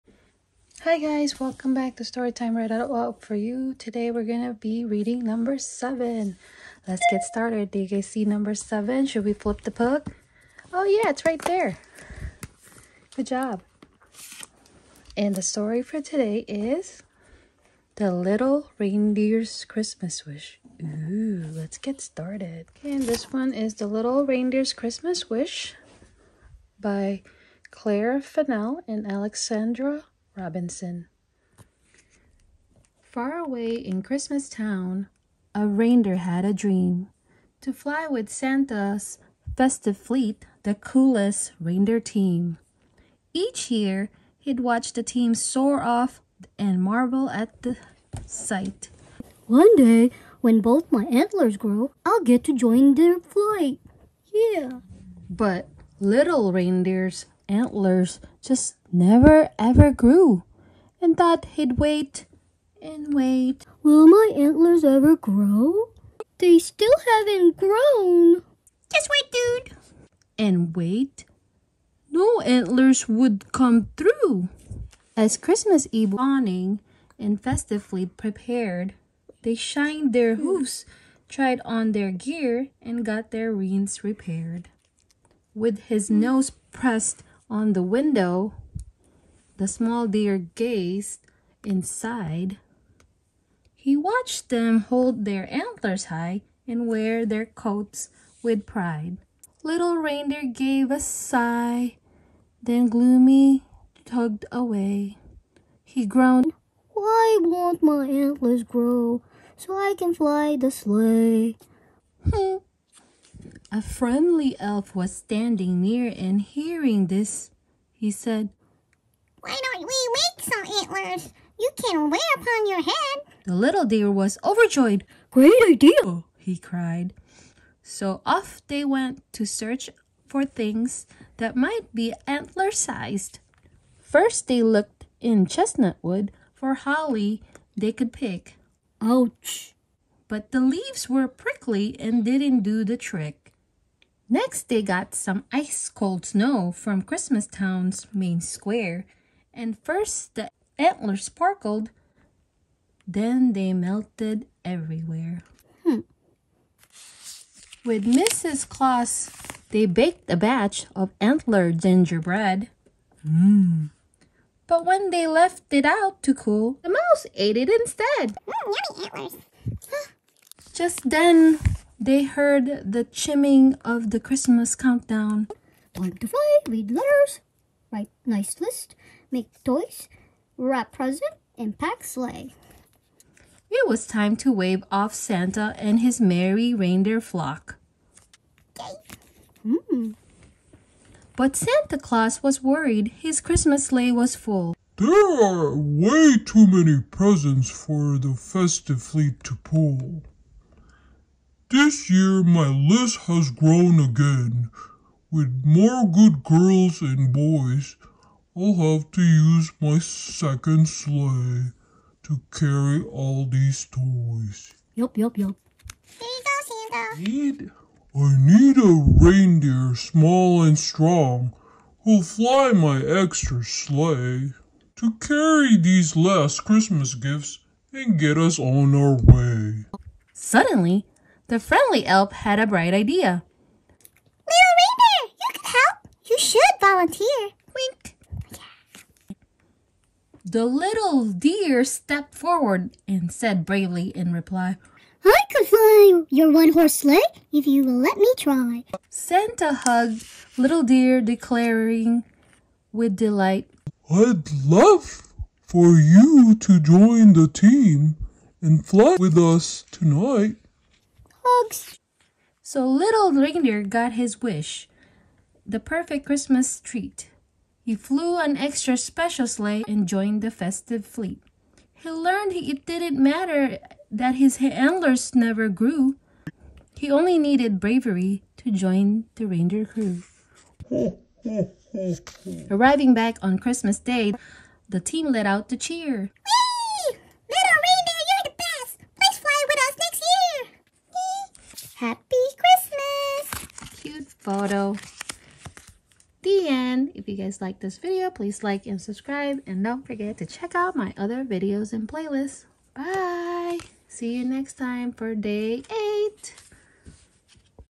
Day 7 | Advent Calendars Holiday Stories | Little Reindeer’s Christmas Wish | StorytimeReadAloud4U
Hi guys!

From the month of December we are reading books as we count down to Christmas with the Advent Calendar Stories - Advent Calendar. Yay!

❤️P.S. You might occasionally hear my kids in the background as at most of the time I’m reading this to them.. the more rhe merrier I say! 😉

It’s Day 7!
The book title for today:  “Little Reindeer’s Christmas Wish”
Read by: Storytime Read Aloud 4u

We hope you enjoyed the story.

See you soon for Day 8 😀

xoxo